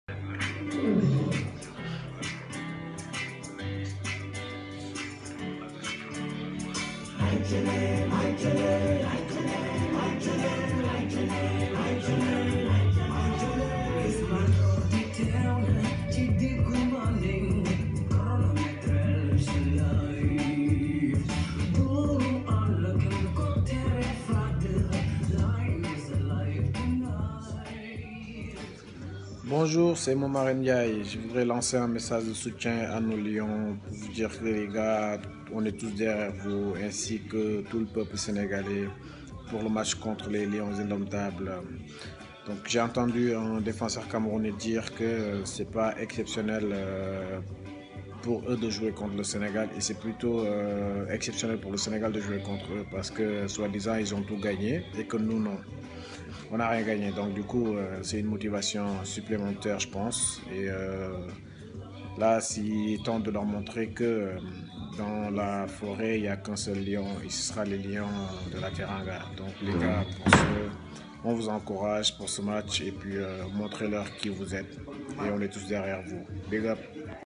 Momar Ndiaye "Tous derrière les Lions"
Momar Ndiaye, ancien joueur du Fc Metz, apporte son soutien aux Lions du Sénégal qui affrontent ce samedi les Lions Indomptables du Cameroun en quart de finale de la CAN 2017.